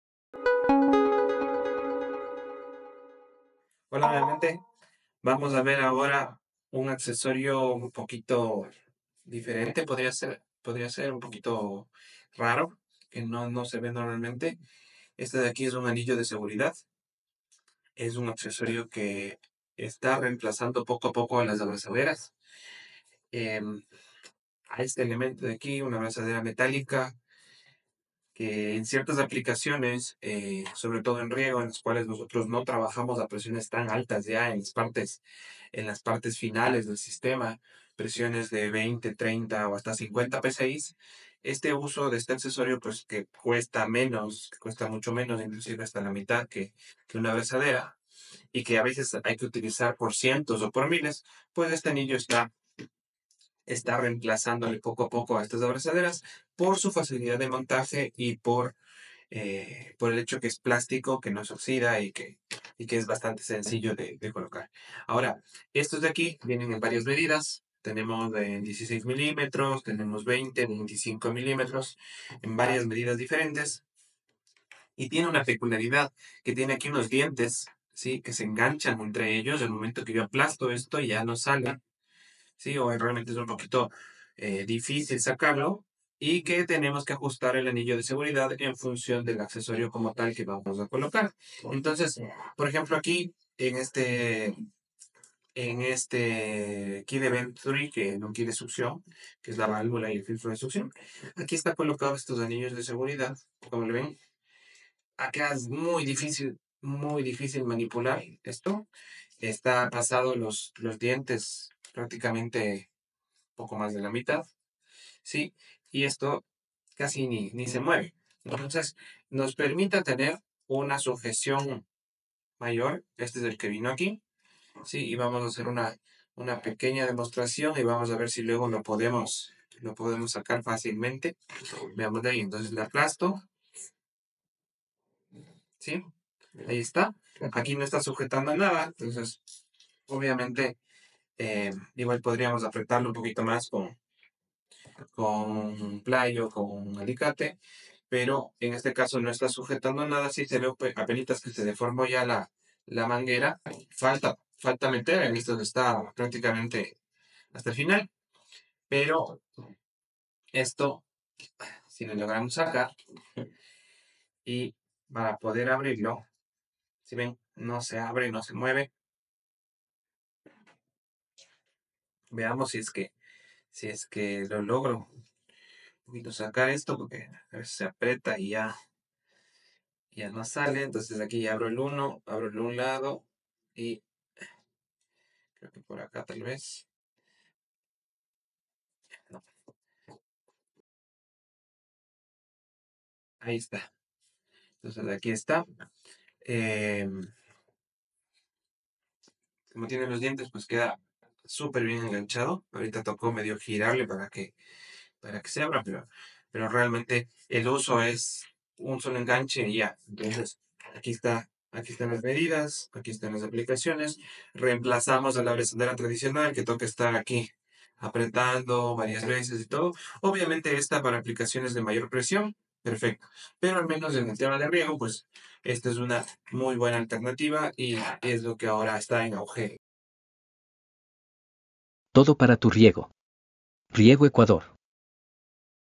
¿Reemplazo de una Abrazadera? Anillos de Seguridad
Un nuevo producto que poco a poco va ganando mercado y se convierte en una solución práctica y de bajo costo para reemplazar a las típicas abrazaderas, sobre todo cuando se manejan presiones no tan altas.